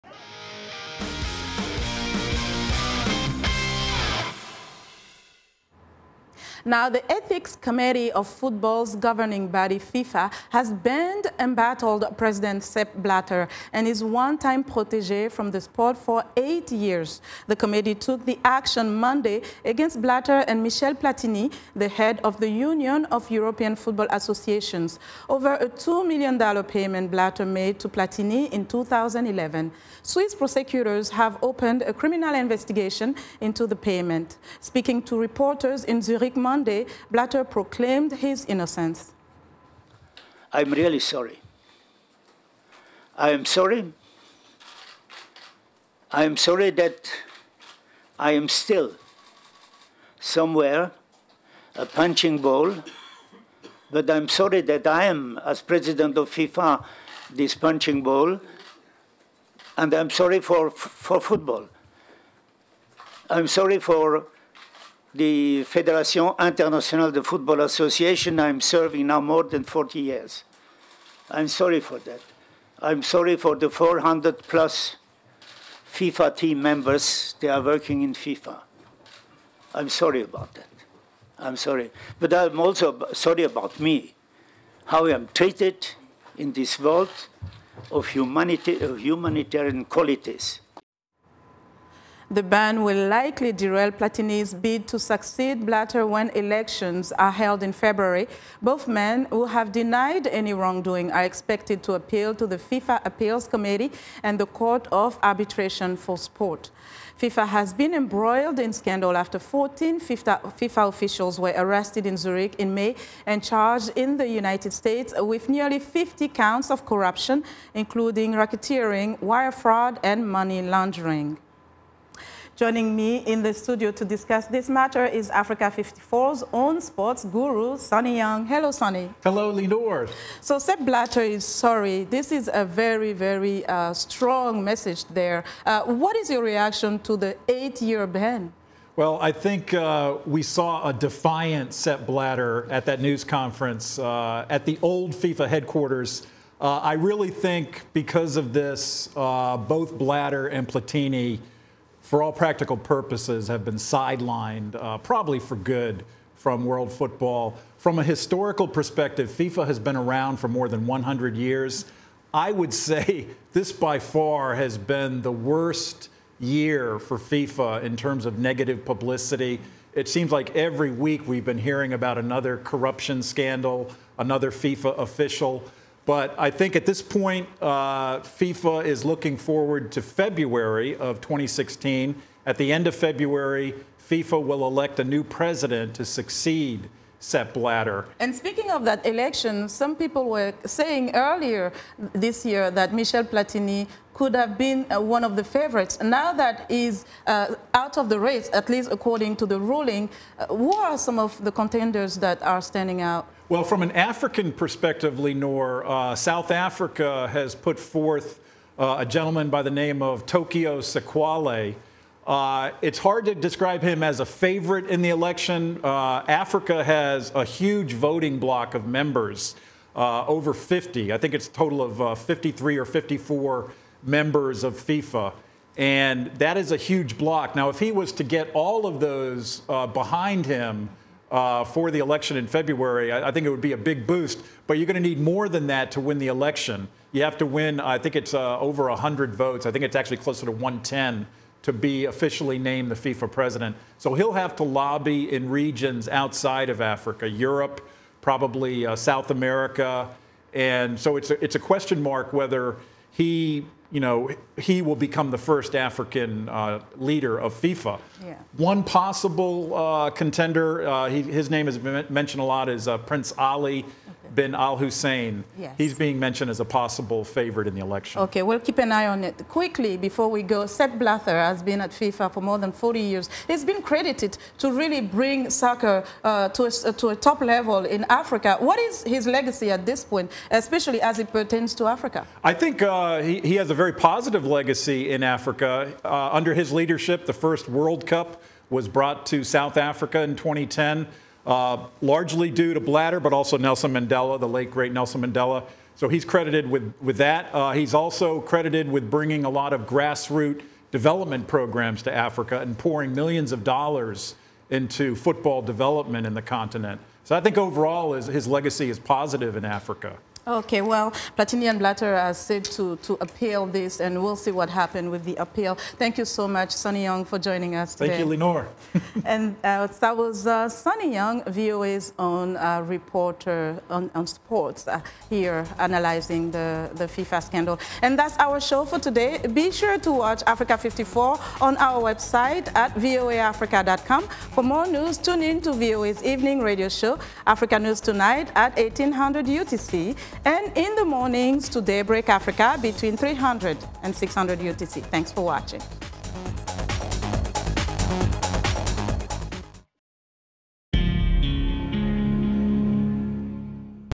FIFA Scandal
VOA’s Linord Moudou reports on the FIFA scandal and later talks to VOA’s Sonny Young, of Sonny Side of Sports, about how the ethics committee of football's governing body FIFA has banned embattled President Sepp Blatter and his one-time protégé from the sport for eight years.